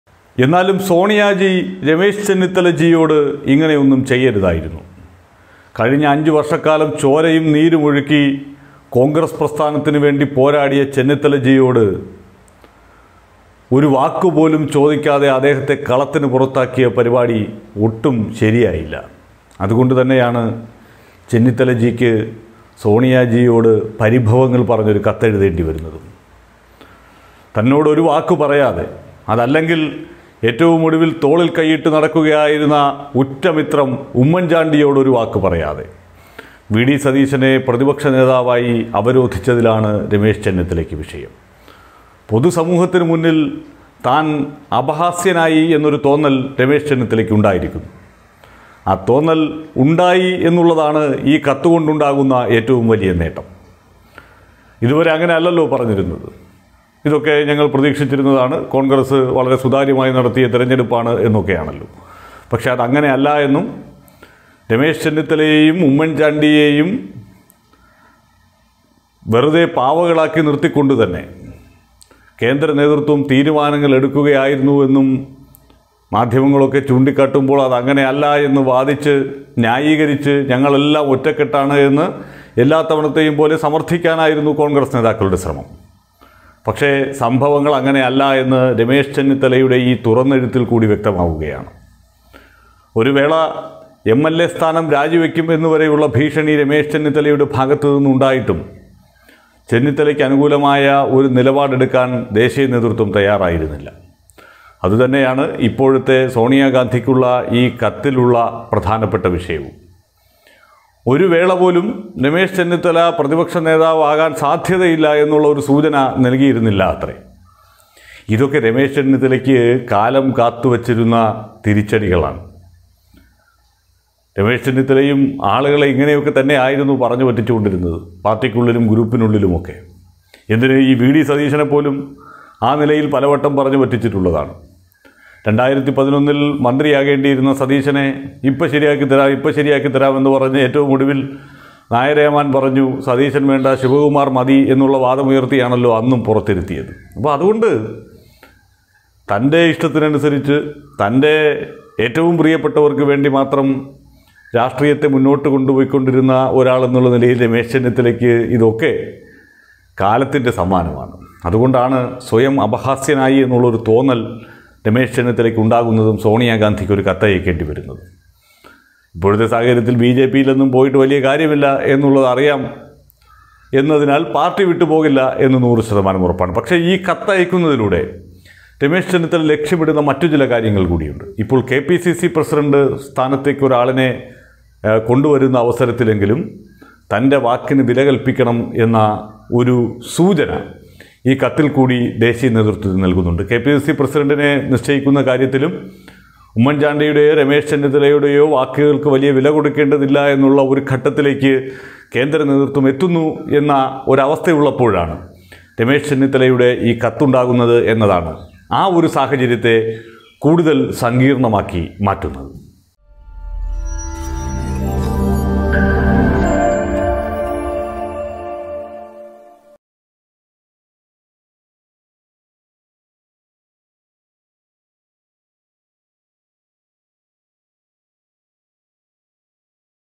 പരിഭവം പറഞ്ഞ് സോണിയക്ക് ചെന്നിത്തലയുടെ കത്ത്‌
ചെന്നിത്തലജീയോട്
ഈ ചതി വേണ്ടായിരുന്നു സോണിയാജീ;
ഒന്നുറക്കെ പറഞ്ഞുകൂടായിരുന്നോ

Tag-Generator-Youtube-malayalam news-global